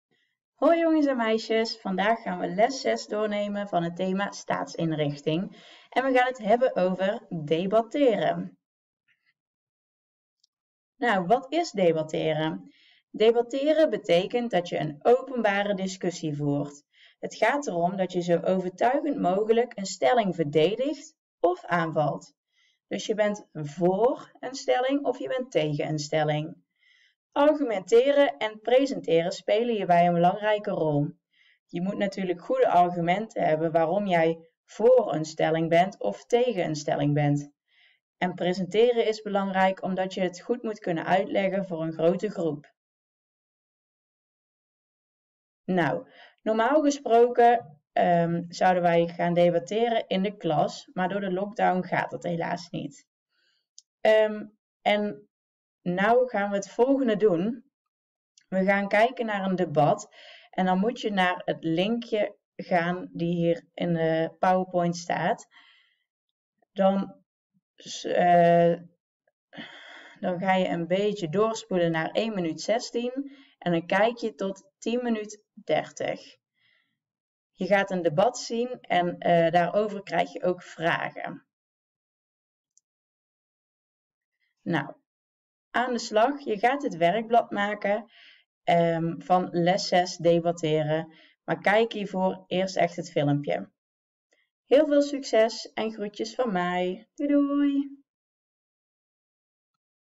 Leerjaar 7 en 8: Debatteren
Weet jij wat debatteren is en hoe je dat moet doen? Luister goed naar de les van vandaag.